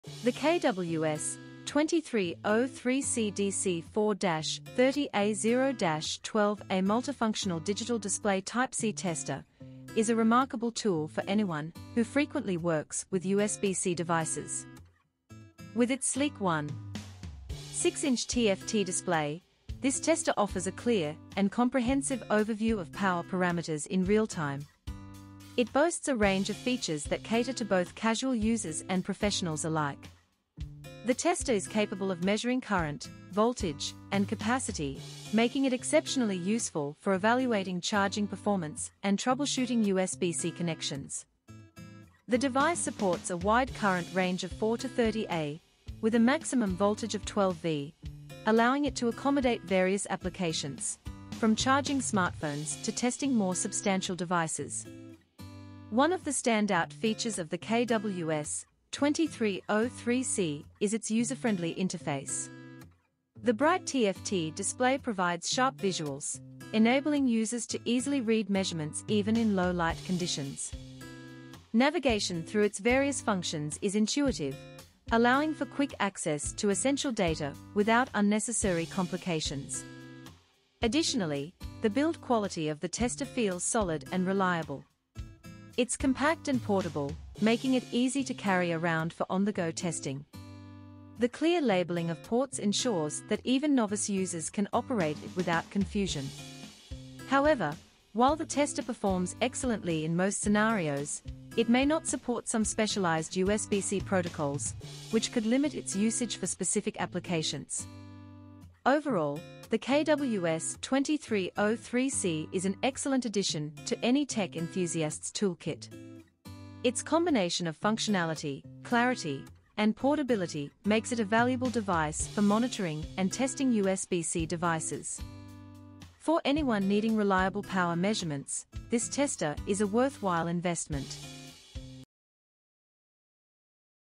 KWS-2303C DC 4-30A 0-12A Multifunctional Digital Display Type-C Tester 1.06 inch TFT Review
The KWS-2303C DC 4-30A 0-12A Multifunctional Digital Display Type-C Tester is a remarkable tool for anyone who frequently works with USB-C devices. With its sleek 1.06-inch TFT display, this tester offers a clear and comprehensive overview of power parameters in real-time. It boasts a range of features that cater to both casual users and professionals alike.

The tester is capable of measuring current, voltage, and capacity, making it exceptionally useful for evaluating charging performance and troubleshooting USB-C connections. The device supports a wide current range of 4 to 30A, with a maximum voltage of 12V, allowing it to accommodate various applications, from charging smartphones to testing more substantial devices.

One of the standout features of the KWS-2303C is its user-friendly interface. The bright TFT display provides sharp visuals, enabling users to easily read measurements even in low-light conditions. Navigation through its various functions is intuitive, allowing for quick access to essential data without unnecessary complications.

Additionally, the build quality of the tester feels solid and reliable. It’s compact and portable, making it easy to carry around for on-the-go testing. The clear labeling of ports ensures that even novice users can operate it without confusion.

However, while the tester performs excellently in most scenarios, it may not support some specialized USB-C protocols, which could limit its usage for specific applications.

Overall, the KWS-2303C is an excellent addition to any tech enthusiast's toolkit. Its combination of functionality, clarity, and portability makes it a valuable device for monitoring and testing USB-C devices. For anyone needing reliable power measurements, this tester is a worthwhile investment.